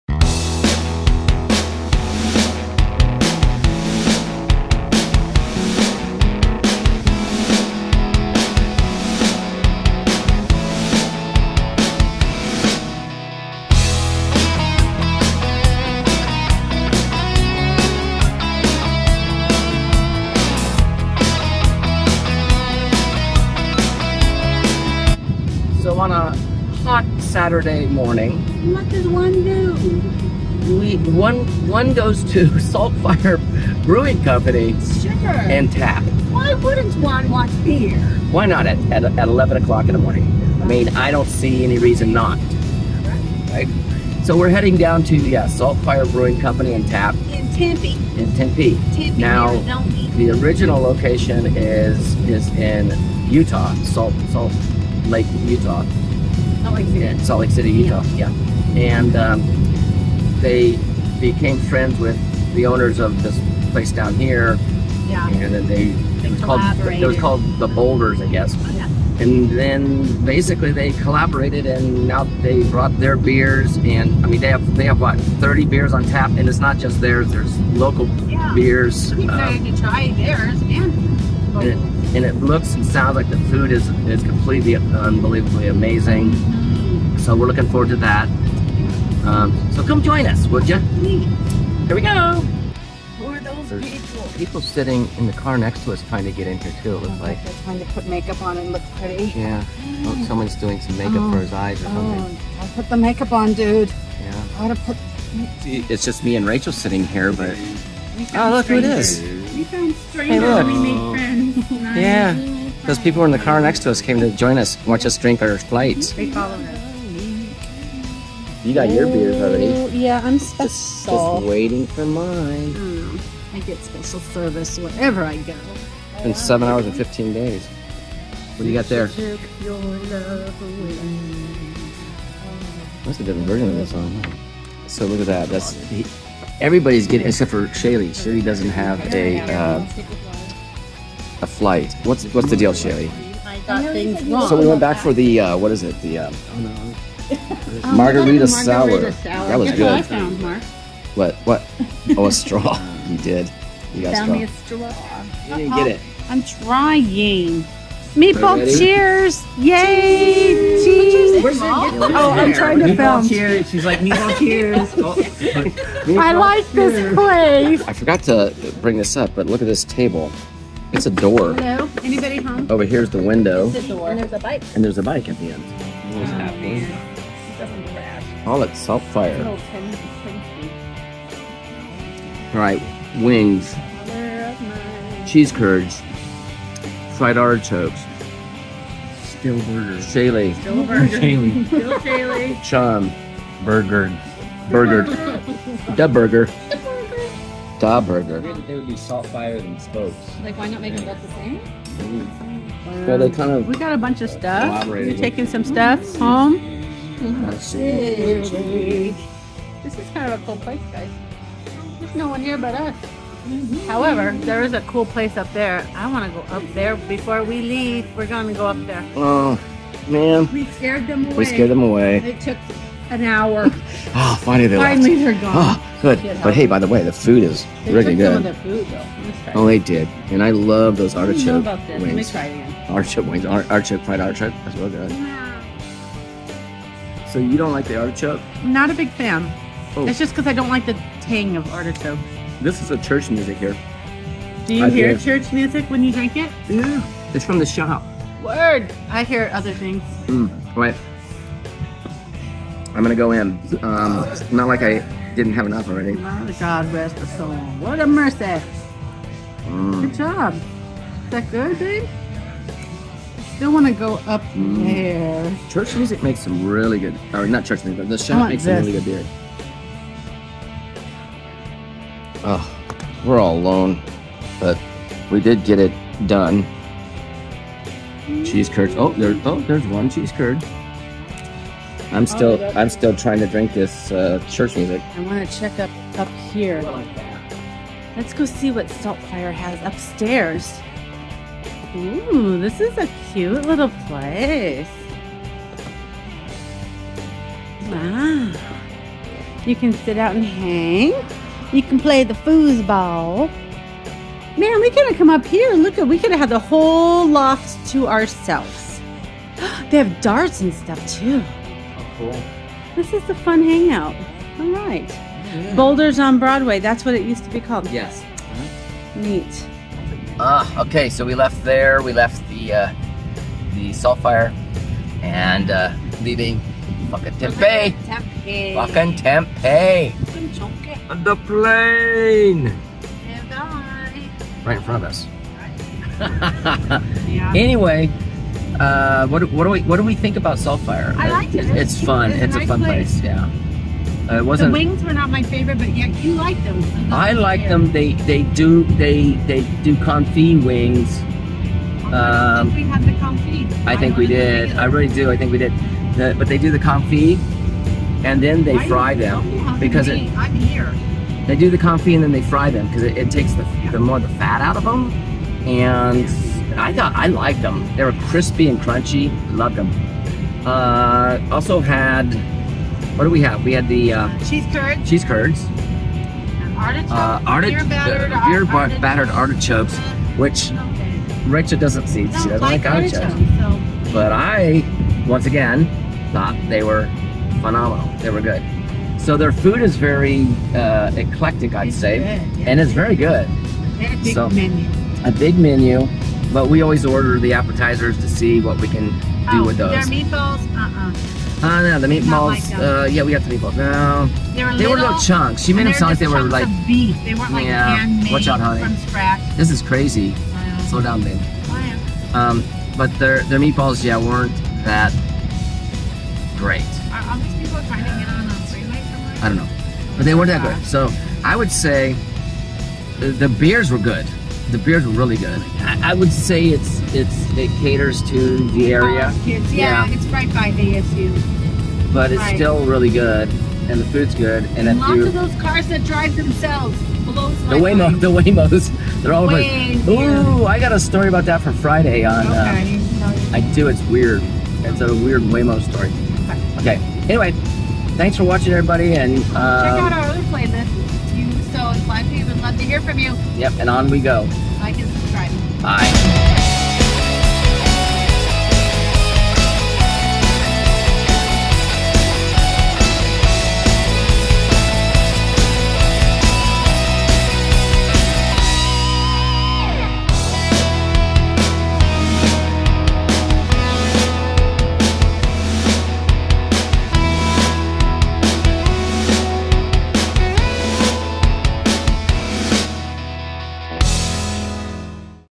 SaltFire Brewery Tap House Tempe Arizona
Mark and Rachel go to SaltFire Brewery and Tap house in Tempe Arizona. They meet up with some special guests to enjoy the flight's and great food! come and join them for this adventure. Cheers!